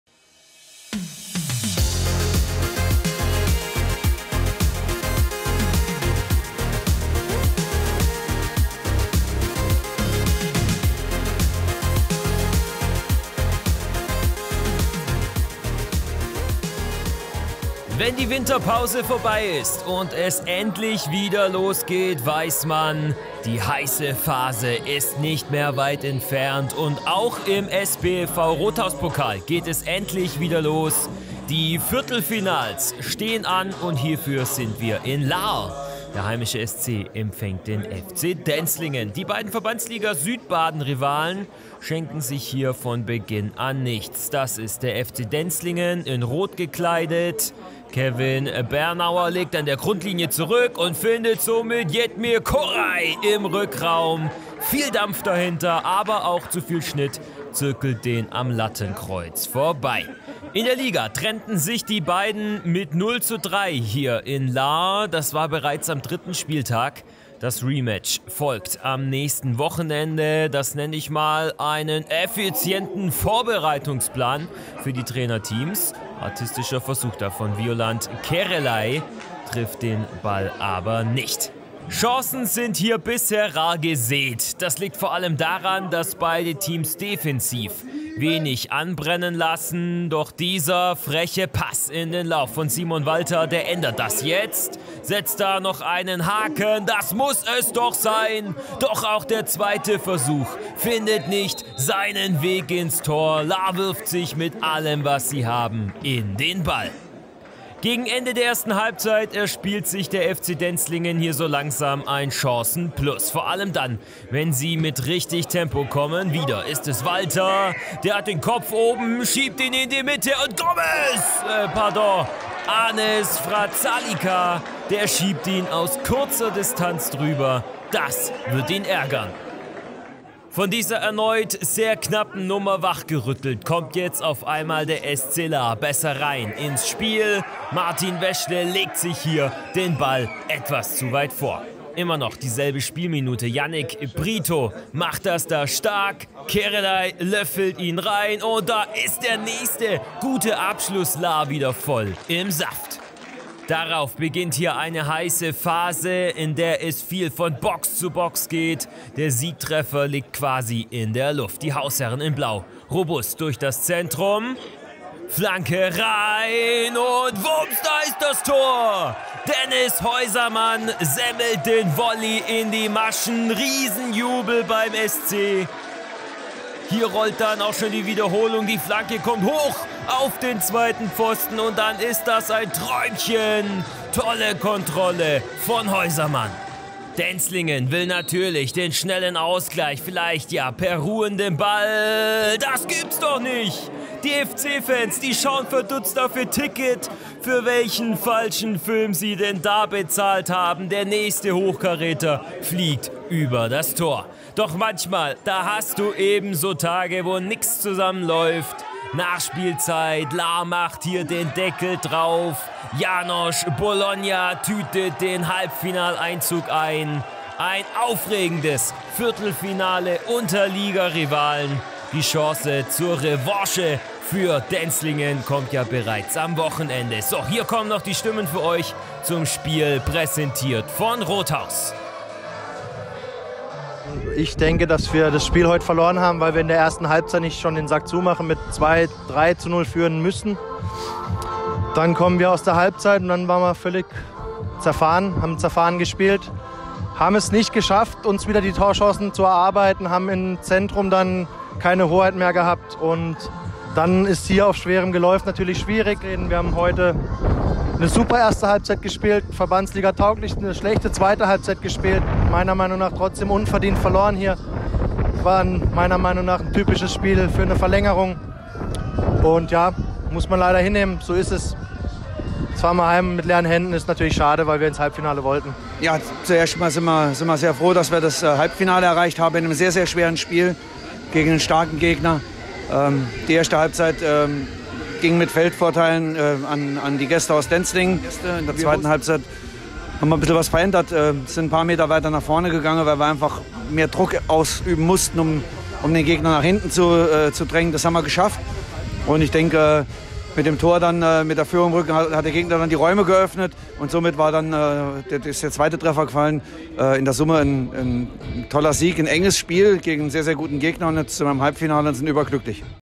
HIGHLIGHTS SC Lahr - FC Denzlingen (Viertelfinale im SBFV-Rothaus-Pokal 2019/20)
Die Höhepunkte der Viertelfinal-Partie SC Lahr - FC  Denzlingen im SBFV-Rothaus-Pokal vom 29. Februar 2020.
Ergebnis: 2:0
⚽ Tore: 1:0 Häußermann (69.), 2:0 Bologna (90.+4)
Zuschauer: 216